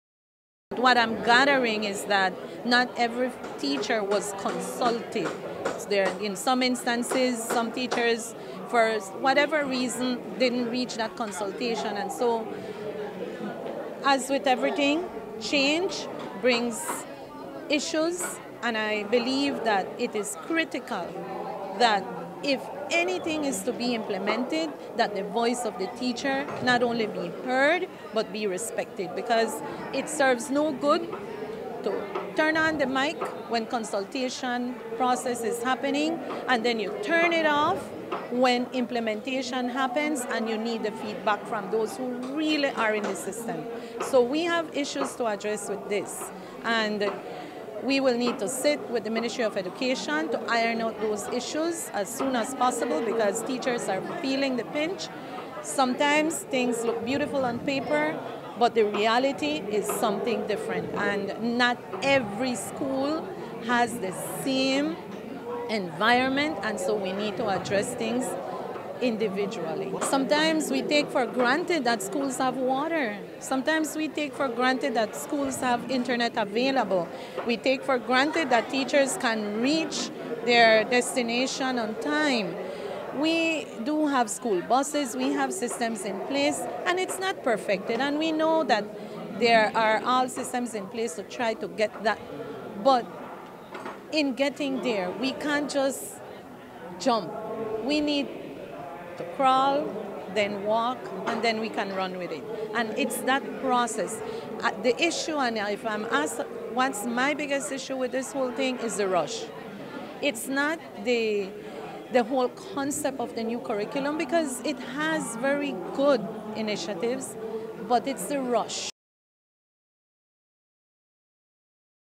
B.N.T.U. President on Curriculum Reform, “Why the Rush?”
President Shoman is also asking the Ministry of Education, why the rush to implement its competency-based curriculum? She argues that some teachers are yet to be consulted on the new curriculum. She further contends that some schools lack basic resources like water and adequate transportation and may not be in a position to take full of advantage of the reform. Here is how she puts it.